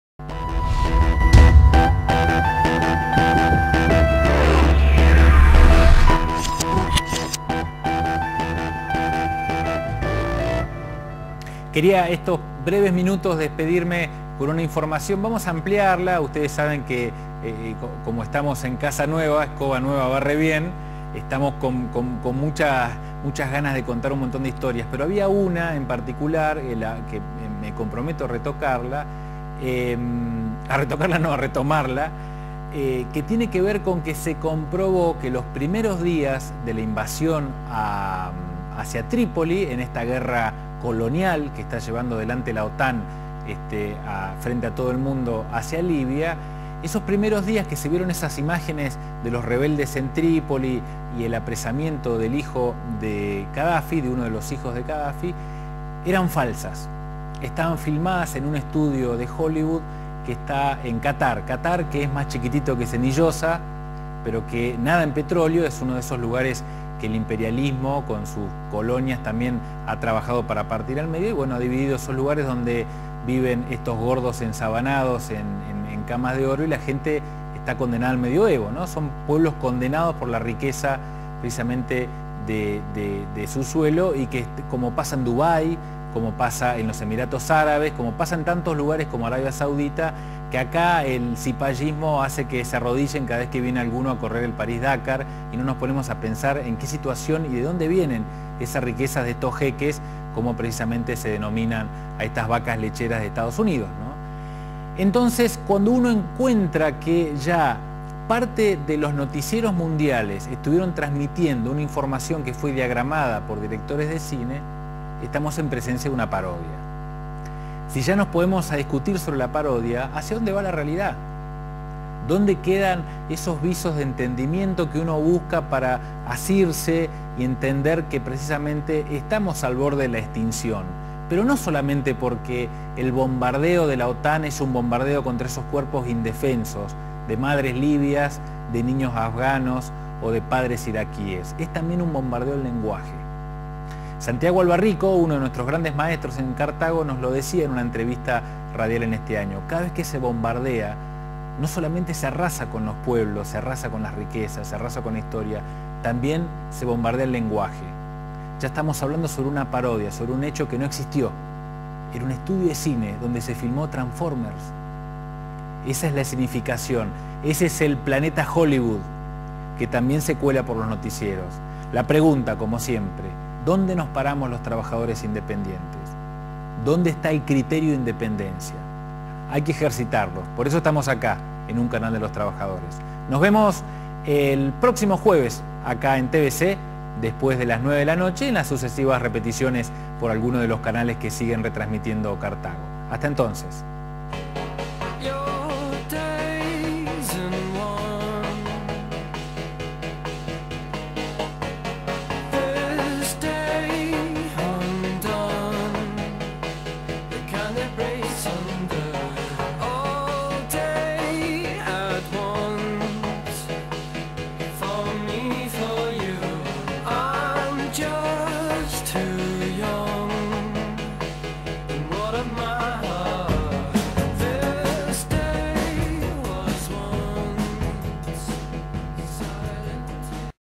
Cartago 2011 Libia: Imagenes falsas de la toma de Tripoli
Líder del CNT admite que imágenes de la toma de la Plaza Verde en Trípoli fueron un engaño
Todos los noticieros emitieron imagenes filmadas en un estudio de cine como si fueran verdad.